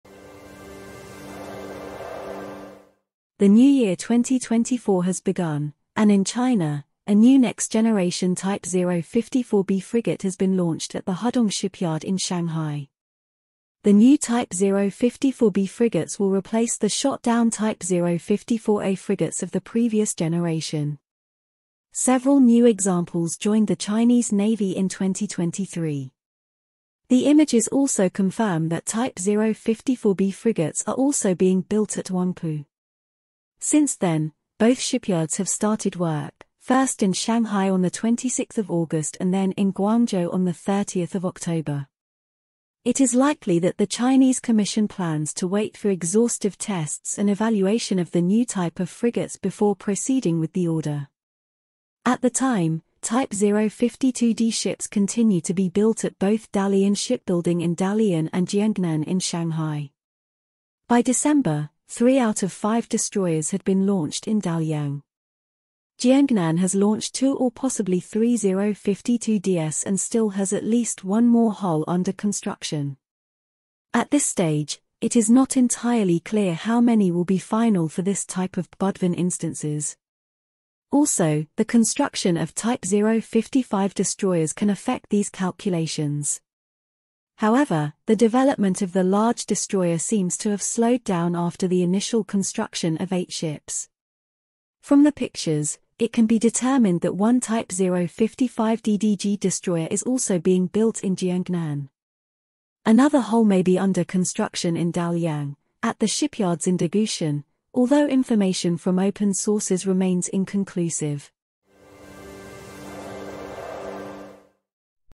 Modernization of the Chinese Navy in 2023: a project 054B frigate was put on the assembly line
USDT: TSJfcHW5UBth2qsR6jYUeGsxJDYYL16YCL

Bitcoin
BTC: 1PYFjXGCBvzyjpbuPRiy1P7DwndWNcTKfG

( Source ):

Type 054B under construction at the Hudong Shipyard in Shanghai. Photo from the network